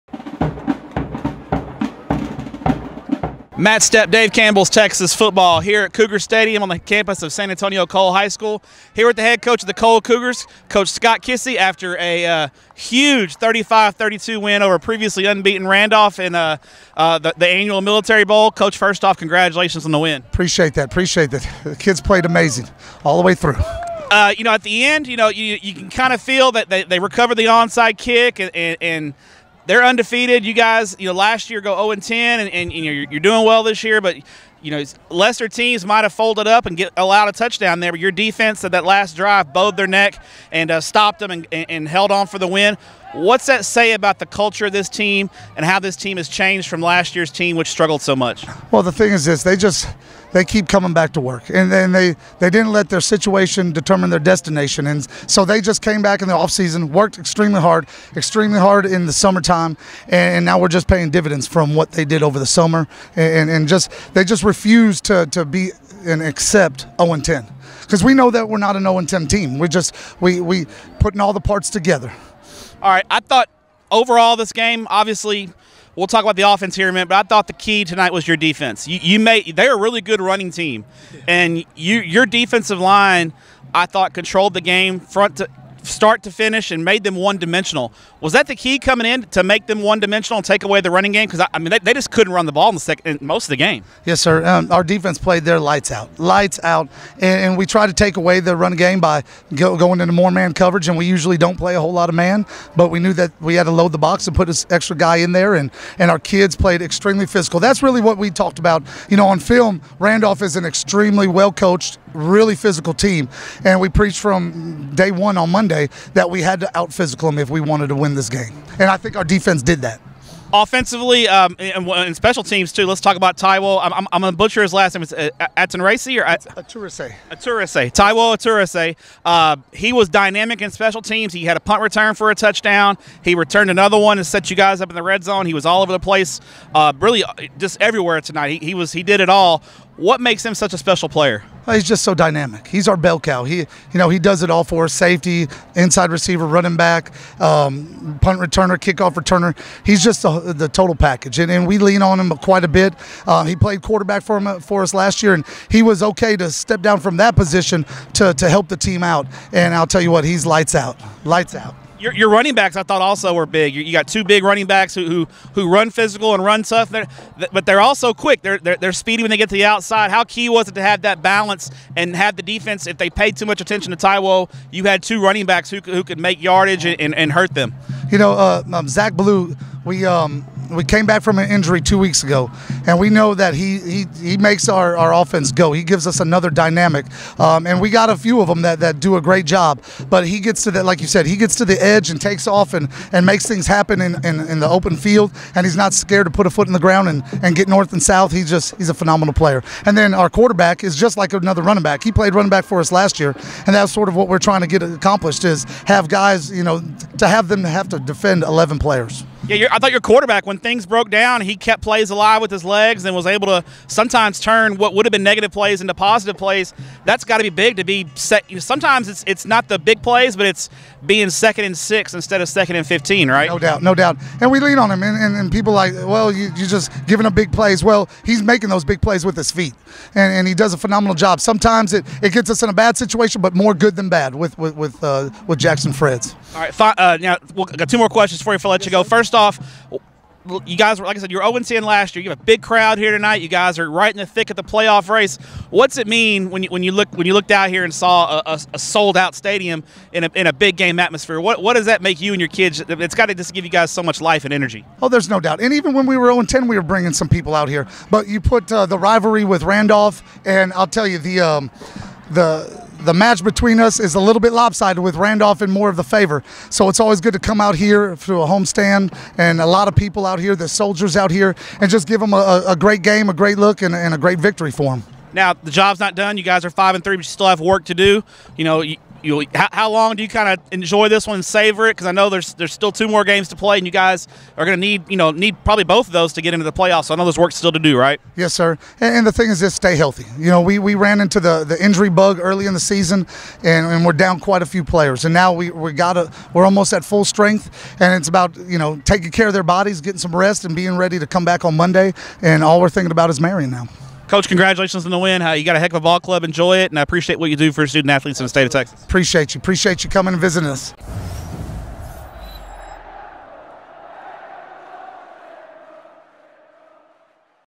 INTERVIEW: San Antonio Cole Cougars head coach Scott Kissee
San Antonio Cole head coach Scott Kissee recaps the Cougars 35-32 victory over the Universal City Randolph Ro-Hawks to move his program to 5-3 in 2023.